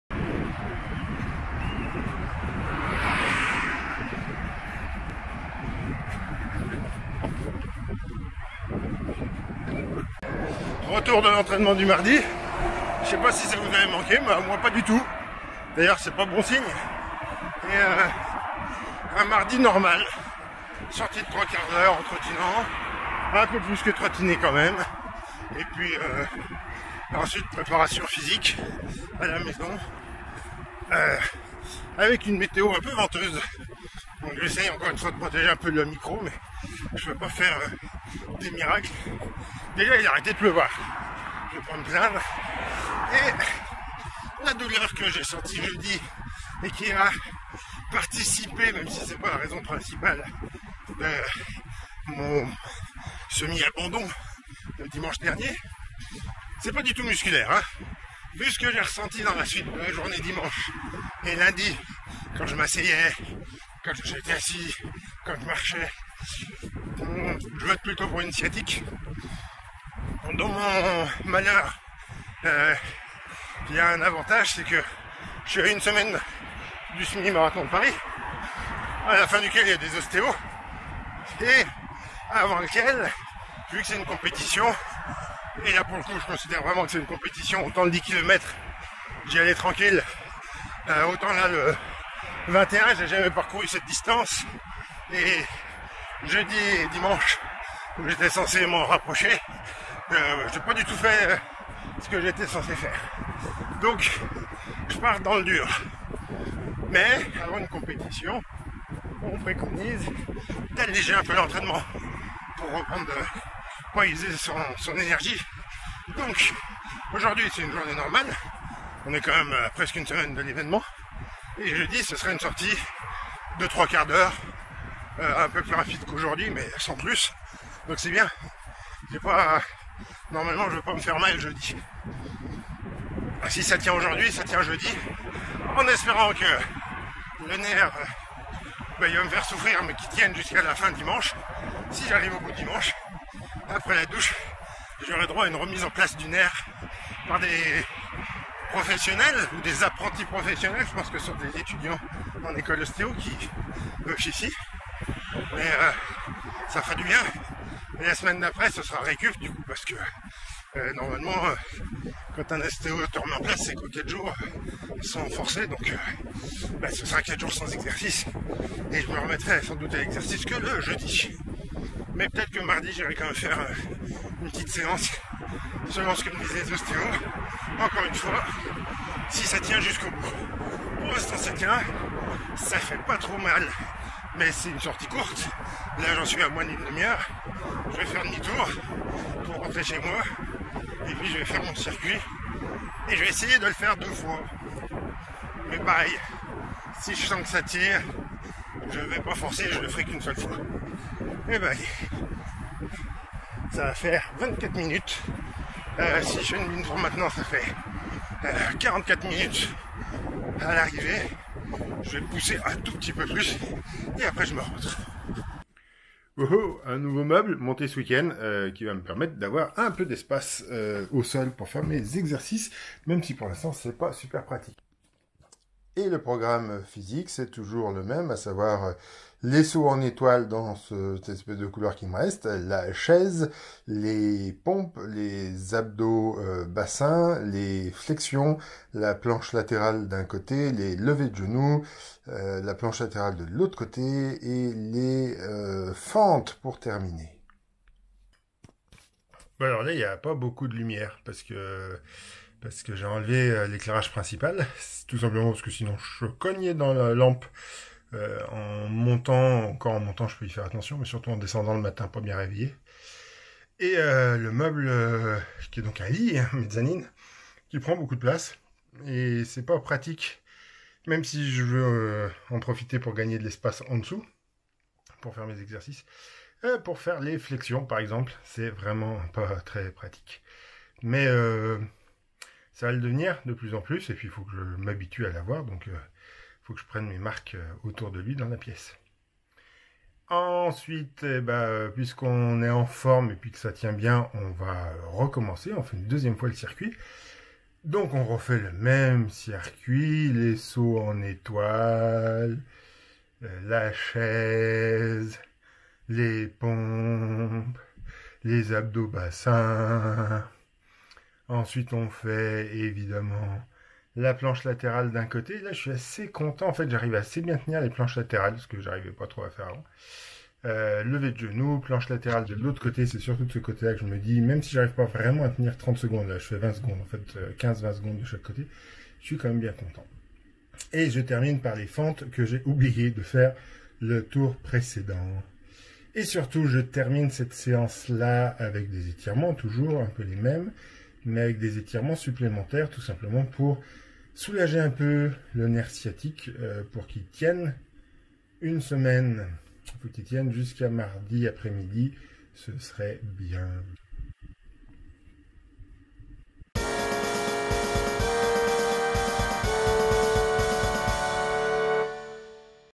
[marathon] s8j1
Retour de l'entraînement du mardi !
45 min en trottinant puis exercices de préparation physique : sauts en étoile, chaise, pompes, abdo bassin, flexions, planches latérales, levers de genoux, fentes.

Si cette vidéo t'a plu :
like la 👍
partage la 📳
🅰️🅱️🅾️nne-toi
commente la 📰
active les notifications 🔔